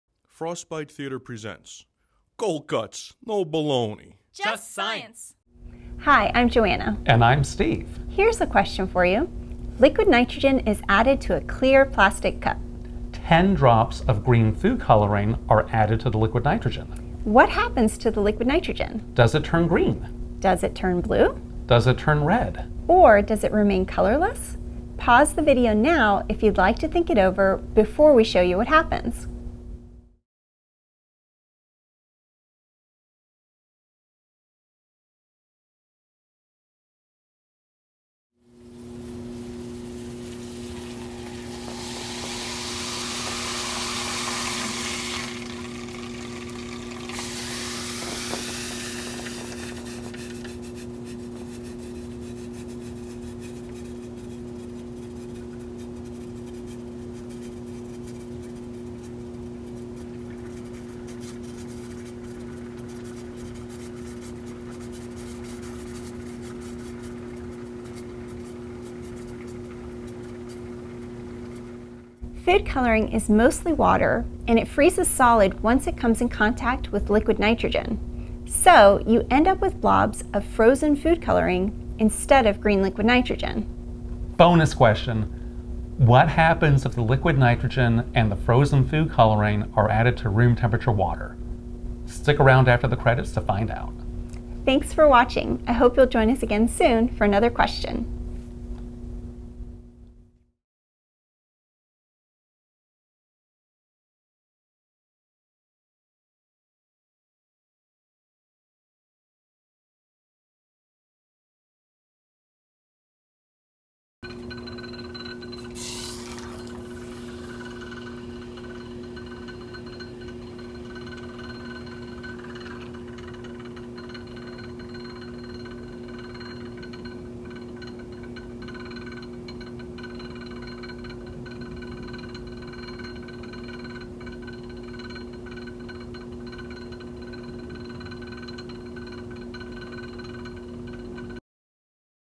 Here's a Question! - Food Coloring and Liquid Nitrogen
Ahhh. The age old question. What happens if you mix food coloring and liquid nitrogen?